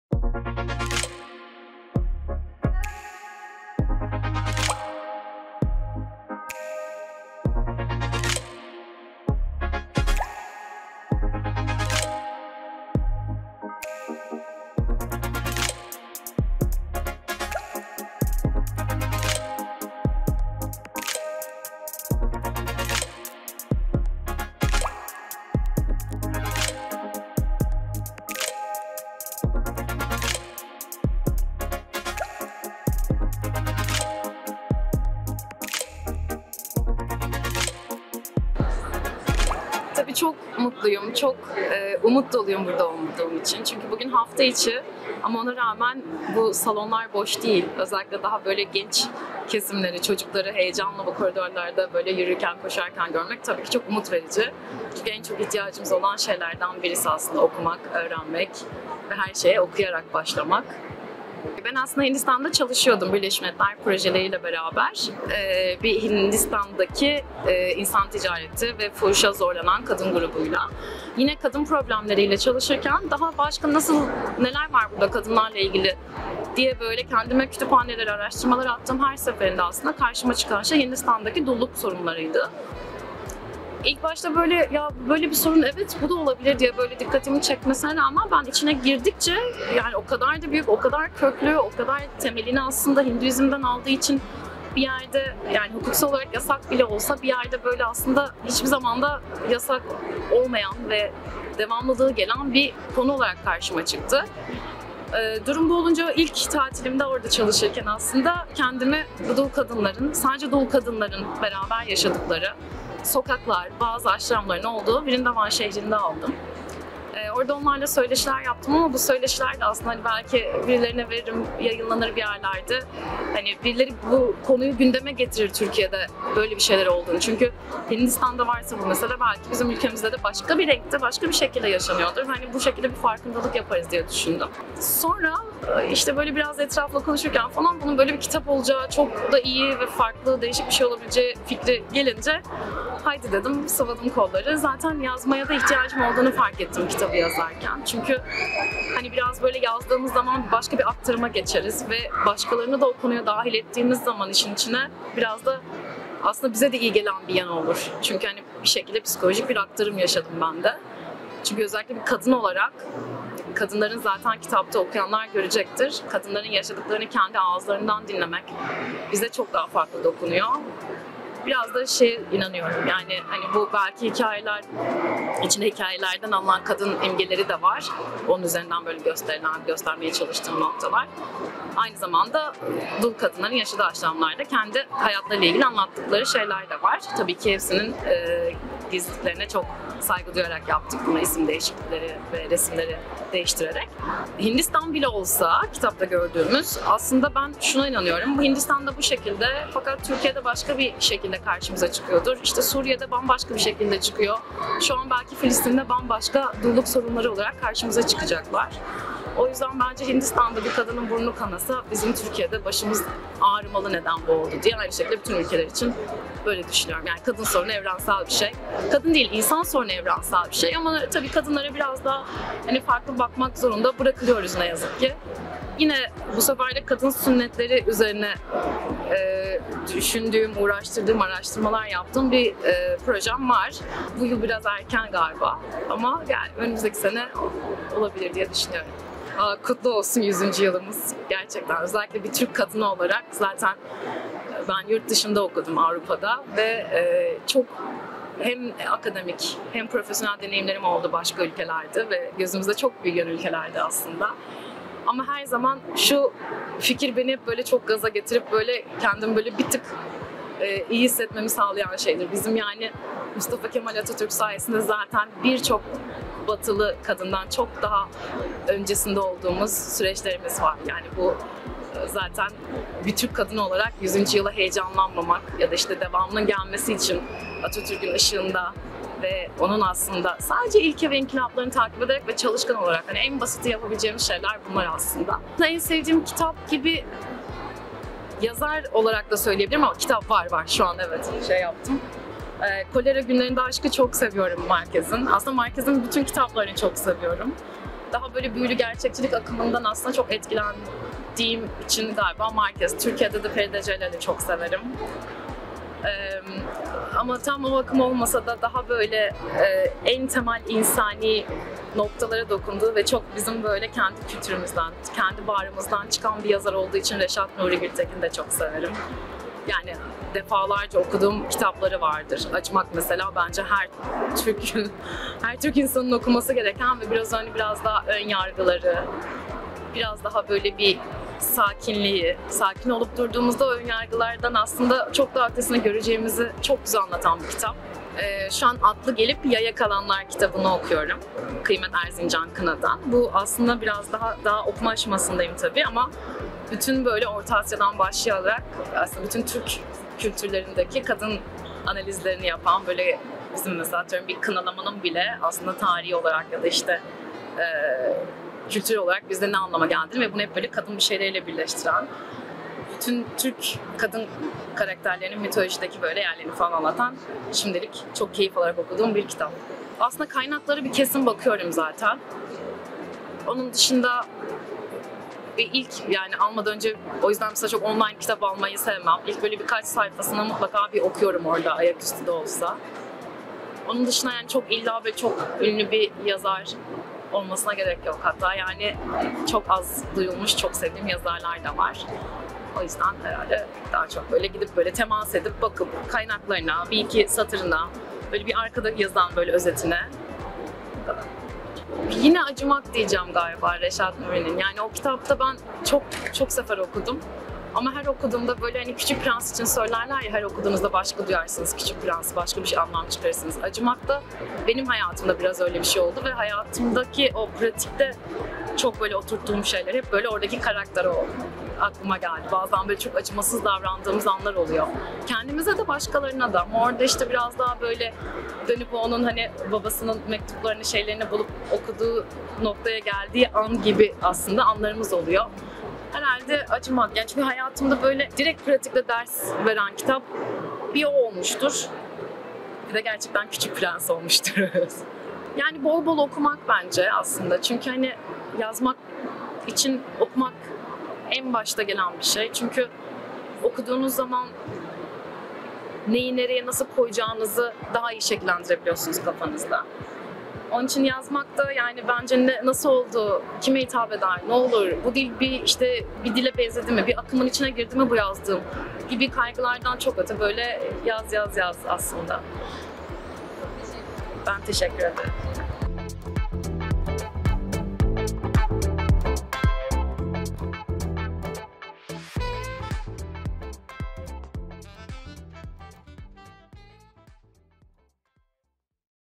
ZEYNEP YAŞAR İSTANBUL KİTAP FUARI’NDA OKUYUCULARIYLA BULUŞTU
Yazarımız Zeynep Yaşar 30 Ekim Pazartesi günü TÜYAP 40. Uluslararası İstanbul Kitap Fuarı’nda Kopernik standımızda imza günü gerçekleştirdi Kendisiyle yaptğımız sohbet yayında! İyi seyirler